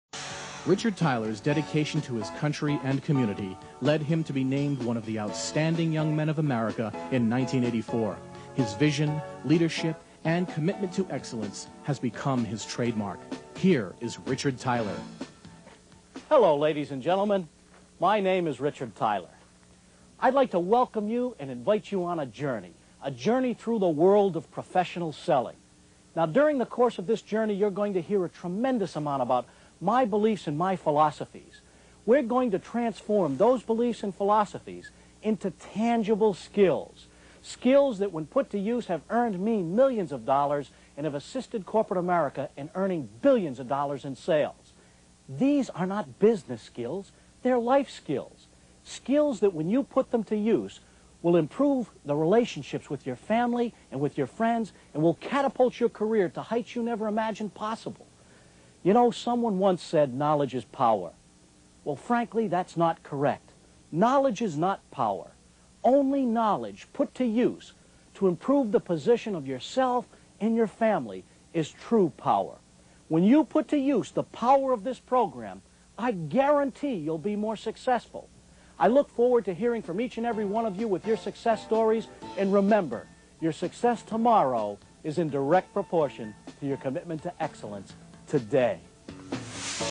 FIRST EVER Richard Tyler International, Inc.® Training Program Promotional Video
Two Richard Tyler and Richard Tyler International, Inc.® firsts!!!

I love this old clip.  The film is a little grainy, as it was shot years ago and in different format.  Back then it was state-of-the-art.   This was the "first ever" promotional video I shot for any of our training programs.  This particular video was produced to introduce students to the very first Richard Tyler "Commitment to Excellence"® Sales Immersion™ Program.

It was filmed in beautiful Newport, Rhode Island over looking the historic Cliff Walk and the Atlantic Ocean.  Can you believe it, I got it in one take.

If you have listened to me over the past 30+ years you will no doubt recognize and appreciate the consistency of the message - Richard Tyler